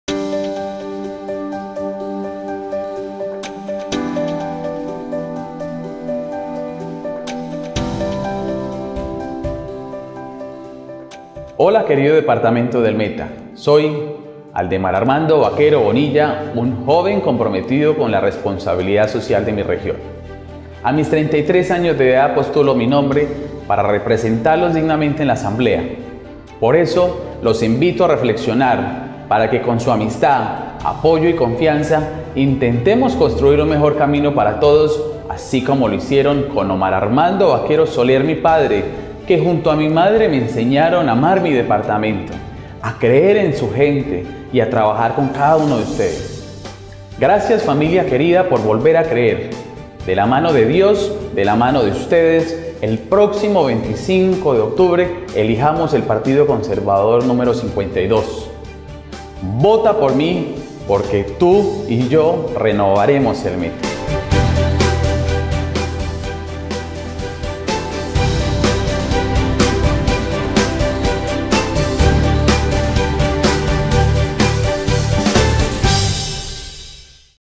Aldemar Armando Baquero Bonilla C52 a la Asamblea por el partido Conservador
Asumo con responsabilidad y amor, el legado que me dejo mi padre, Omar Armando Baquero Soler. Servir de forma altruista, dedicando cada segundo de mi vida en pro de la sociedad metense, Por que llego la hora de renovar el Meta, participa con tu comuna y se parte de caminoparatodos.